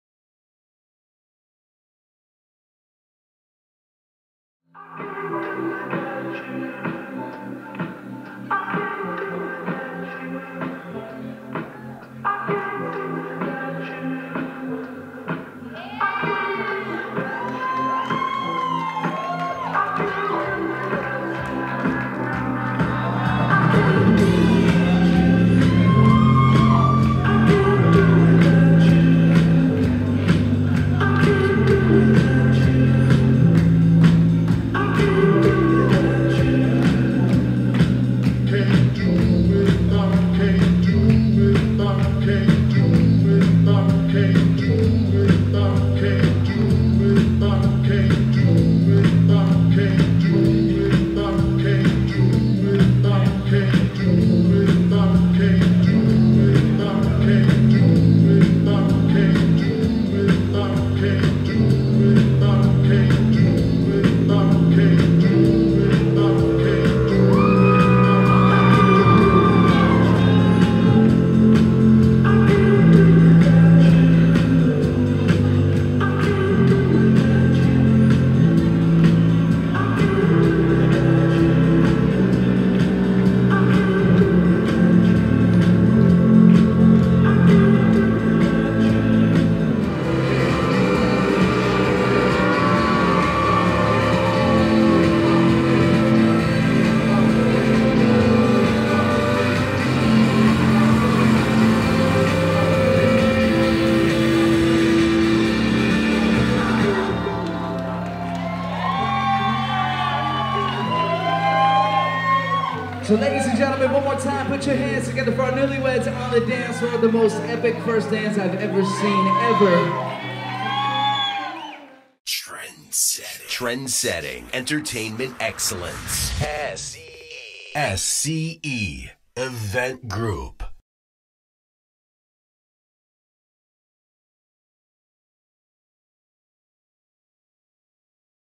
The Bartletlett First Dance Lighting Demo | Christian LaGrotteria | SCE Event Group
Here is an awesome first dance lighting demo that Josh Christie from the SCE Event Group constructed specifically for their first dance. This included 8 moving dance floor lights and an upgraded color control uplighting package. DJ Christian LaGrotteria supplied the sound track all night long.